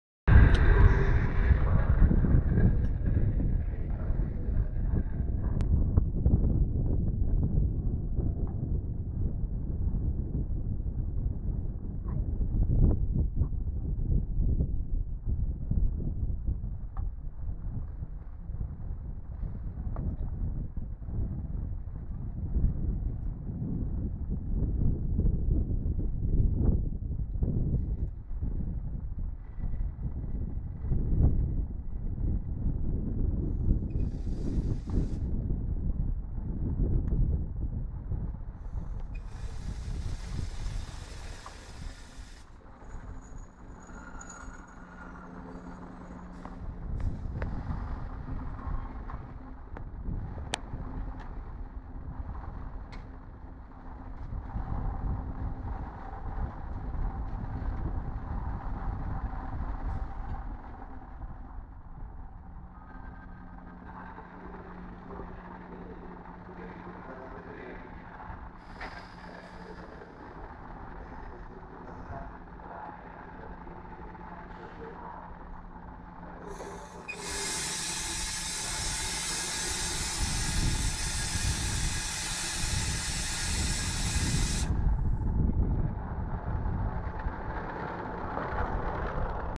Arrivo a Roma Termini di una tripletta di Aln 663 proveniente da Campobasso
Binario 20 bis, stazione Termini. Regionale 2359 Campobasso - Roma Termini.